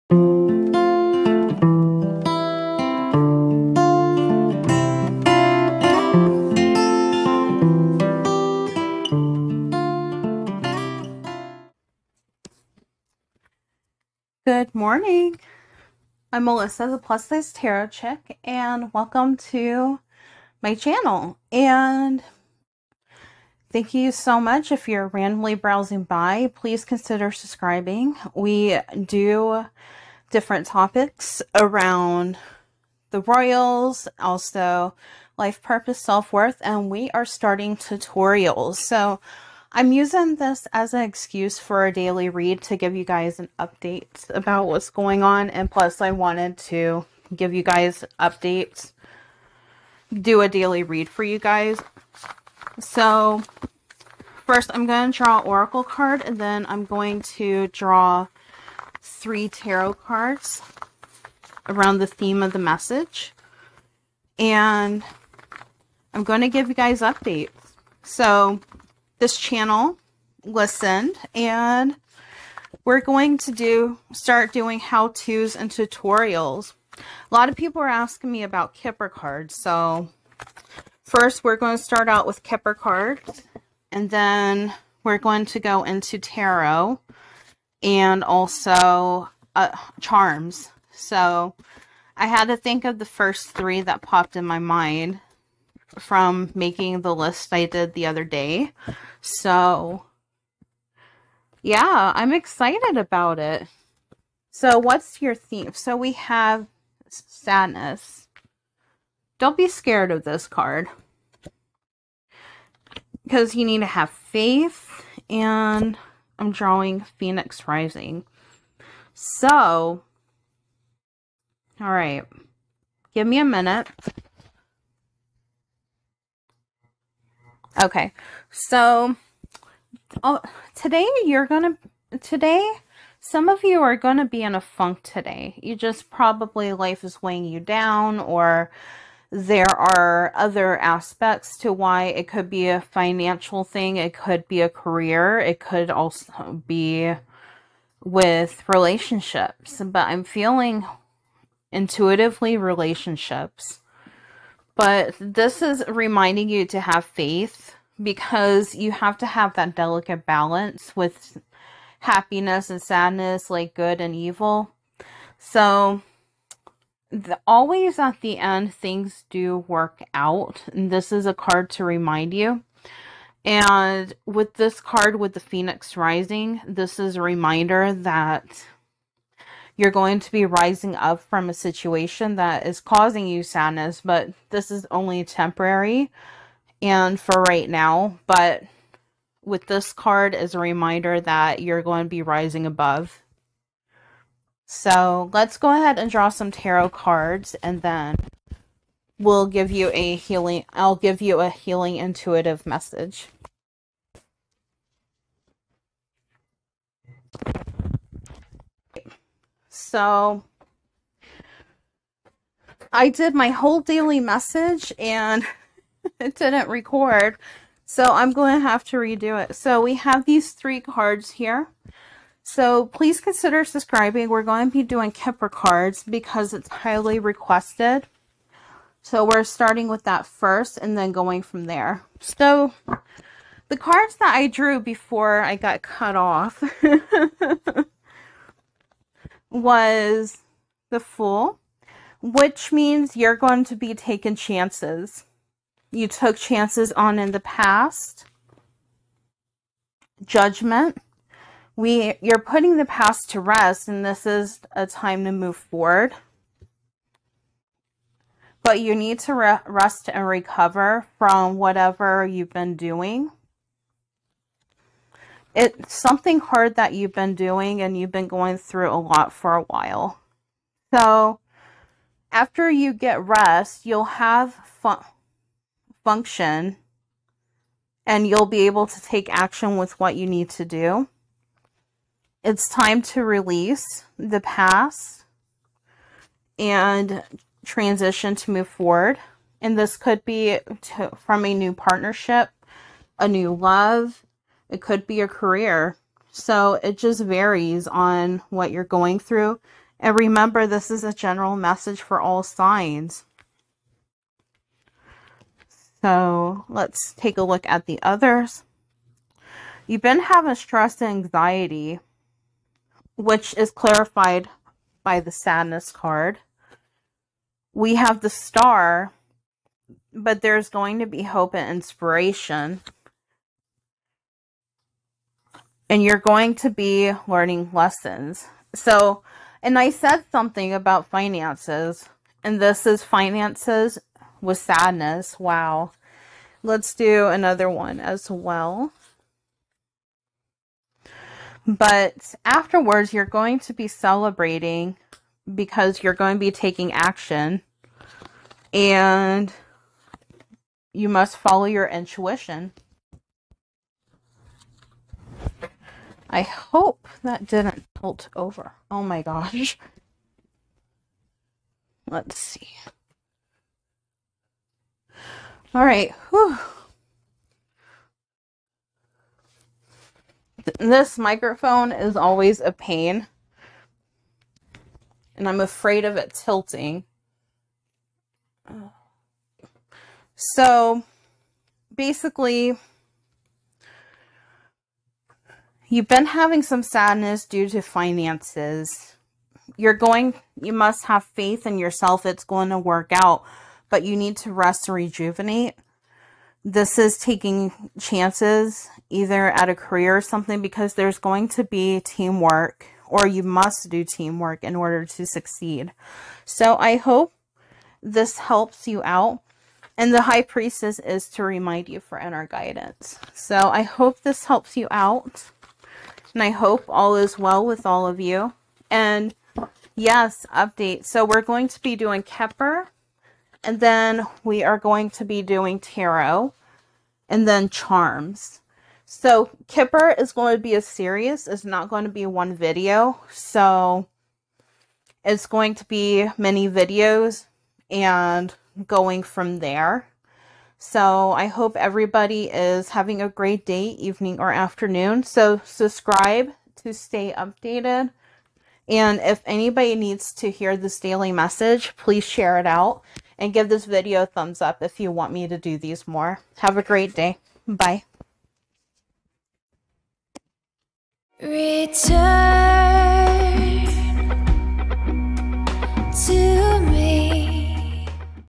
Today's Tarot Message and The Plus Size Tarot Chick Channel Updates
Get the tarot forecast today with this daily tarot reading with oracle cards and tarot cards. WIth tarot guided message and the card of the day.